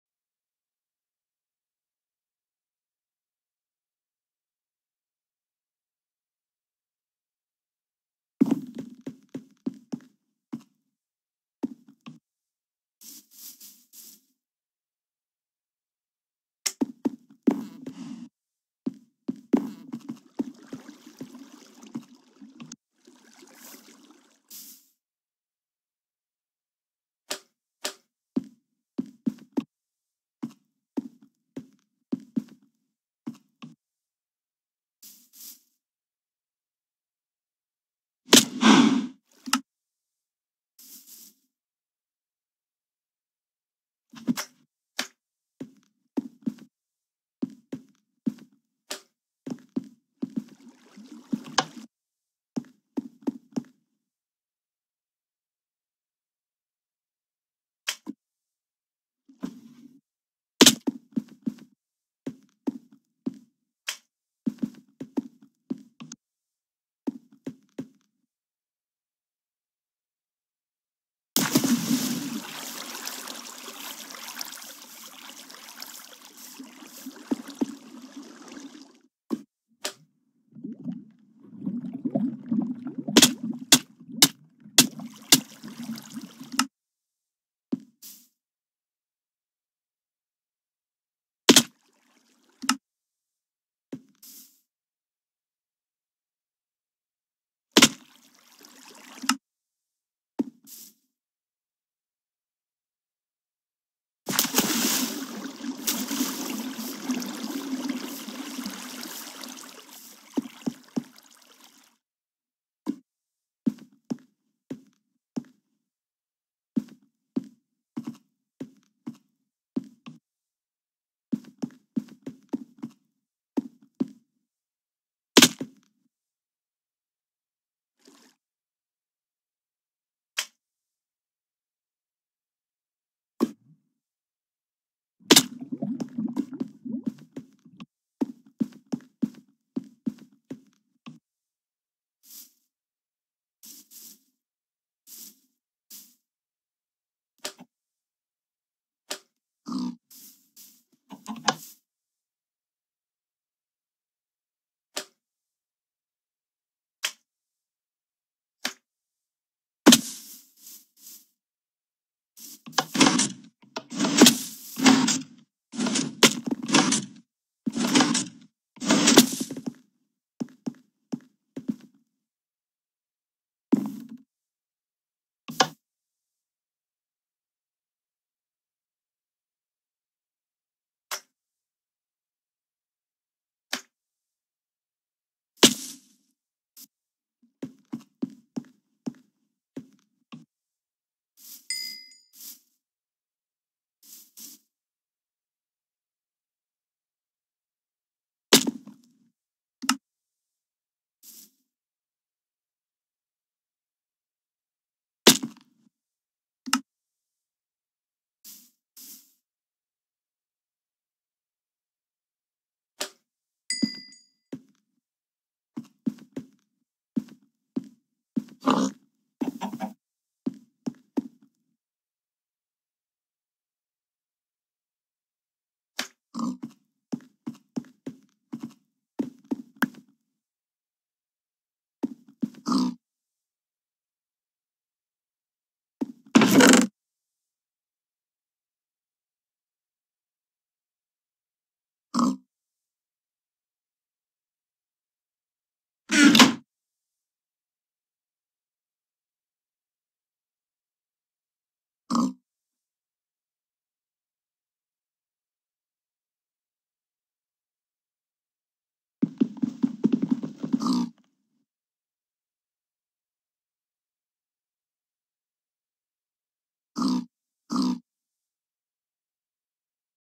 Freefall speedrun: 3:43
In this video I do the map freefall. This time I have a twist on it! I will complete it in the quickest amount of time! My time was 3:43